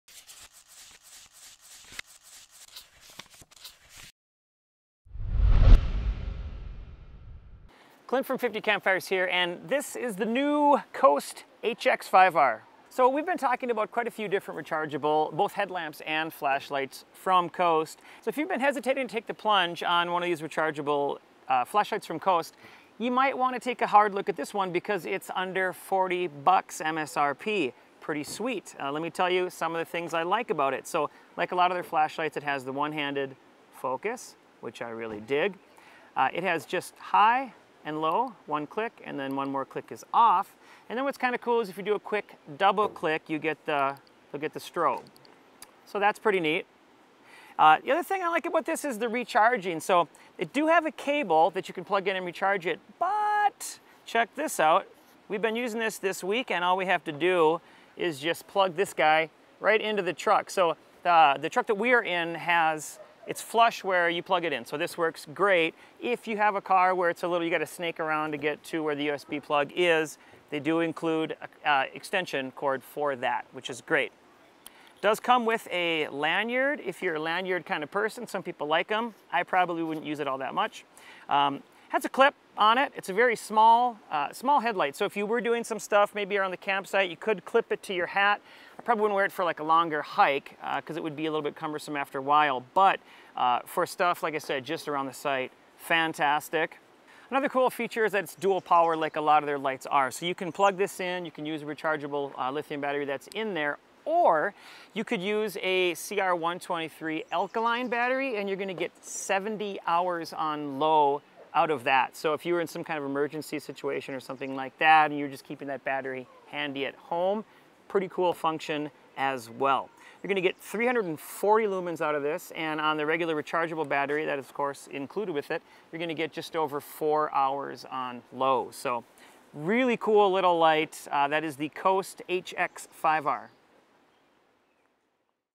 Coast HX5R Flashlight Review : Compact Rechargeable LED Flashlight
COAST HX5R : FLASHLIGHT
USB rechargeable flashlights are all the rage … and for good reason. They work well time after time.The only drawback is they can be a substantial investment. The COAST HX5R solves that problem.

For $40 MSRP, it delivers the convenience and reliability of higher-priced rechargeables plus a substantial list of “must have” features. That includes one-handed focus, one-click high and low beams, and a signaling strobe. It comes with a recharging USB cable, but just as easily plugs directly into any USB port in your vehicle or anywhere else.

Operates off either the rechargeable lithium ion battery (included) or CR123 alkaline battery.